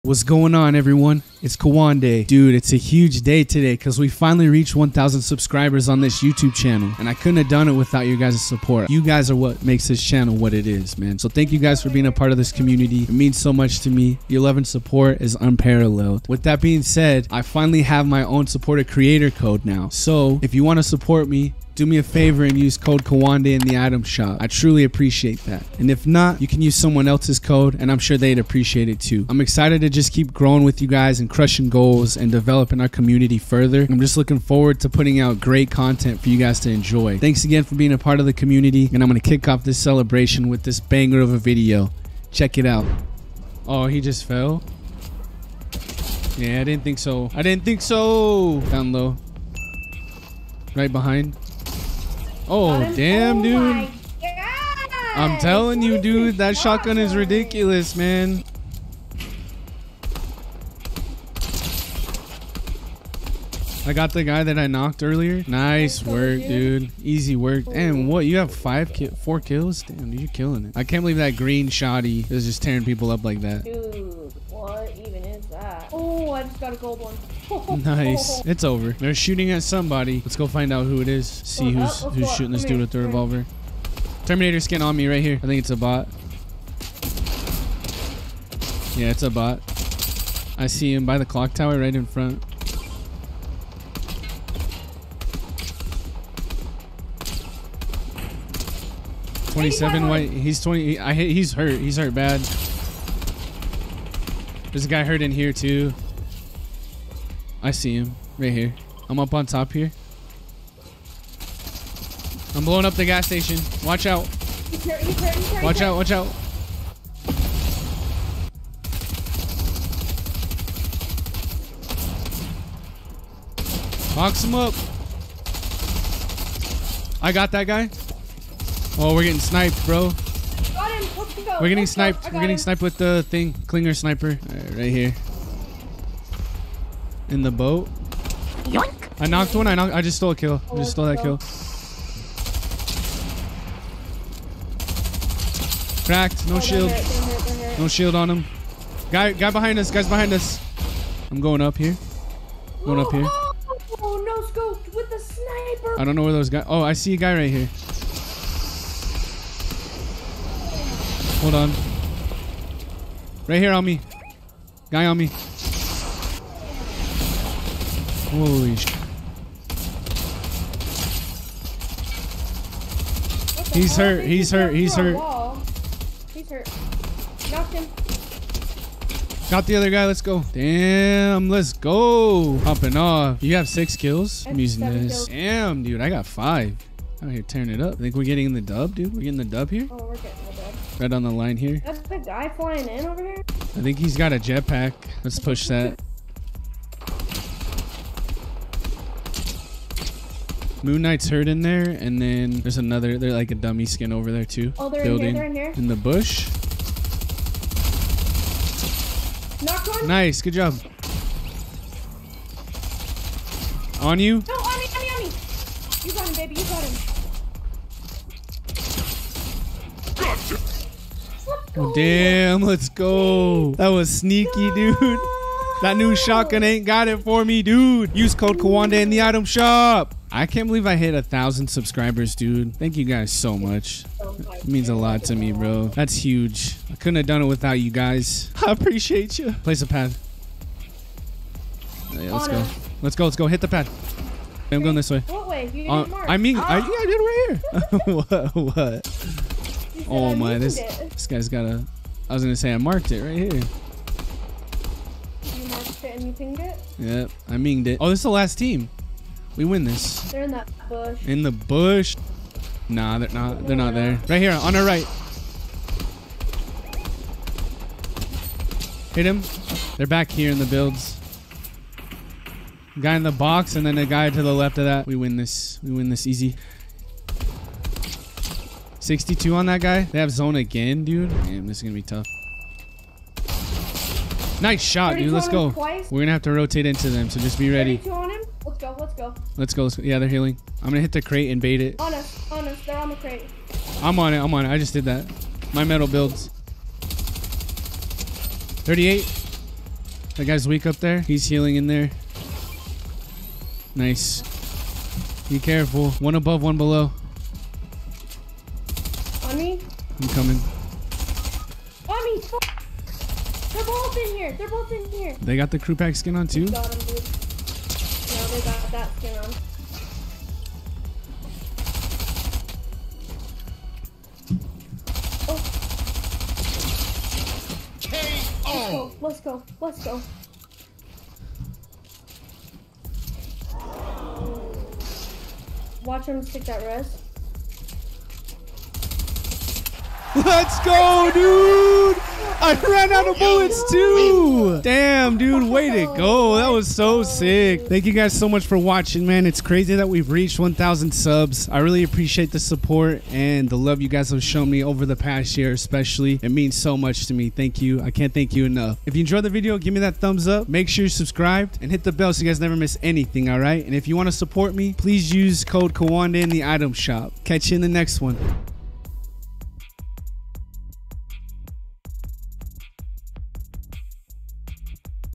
Thank You For 1000 Subscribers! I Have A Fortnite Creator Code Now
We've come such a long way and I'm excited to keep growing with you all. Here's to all of you subscribers old and new and to everyone who enjoys Fortnite as much as I do.